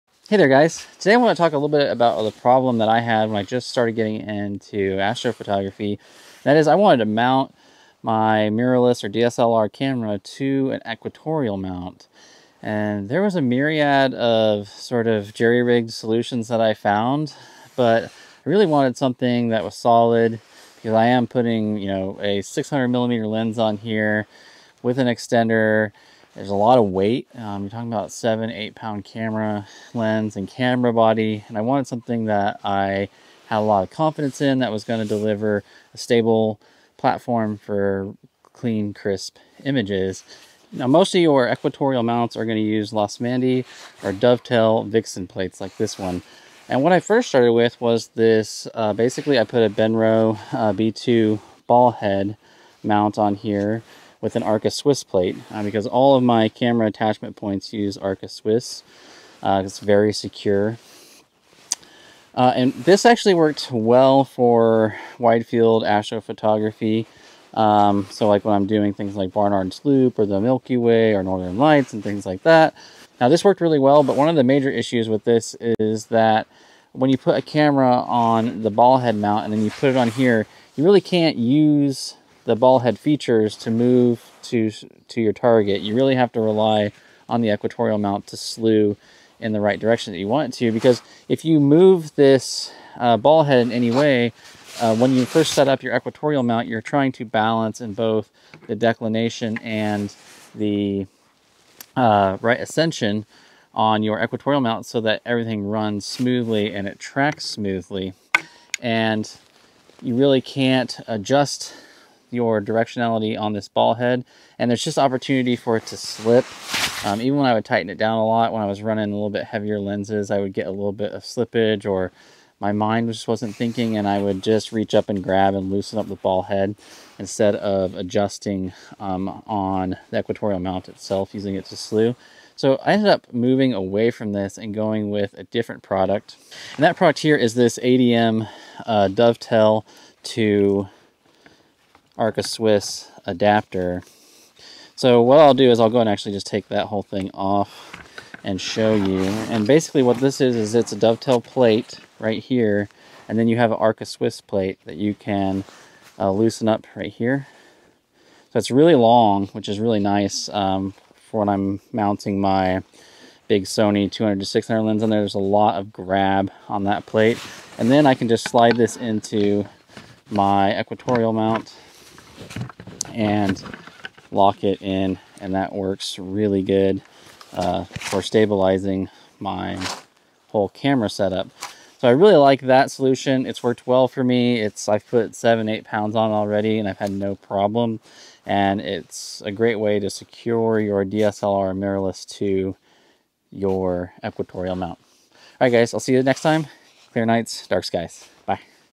Mounting a DSLR or Mirrorless Camera on an Equatorial Mount
When I first got into astrophotography I knew I would need a star tracker or equatorial mount to capture photos of deep sky objects. I ended up going with an equatorial mount so that I could ultimately grow into the hobby with a refractor telescope. However, I wanted to start with using DSLR or mirrorless cameras on the equatorial mount but finding ways to mount them were problematic.

Initially I started with a ball head mount secured on a Vixen dovetail plate but finally ended up settling on the larger and more secure ADM Vixen dovetail to Arca-swiss adapter. Hope this helps!

Products featured in this video:
- Benro B2 Ballhead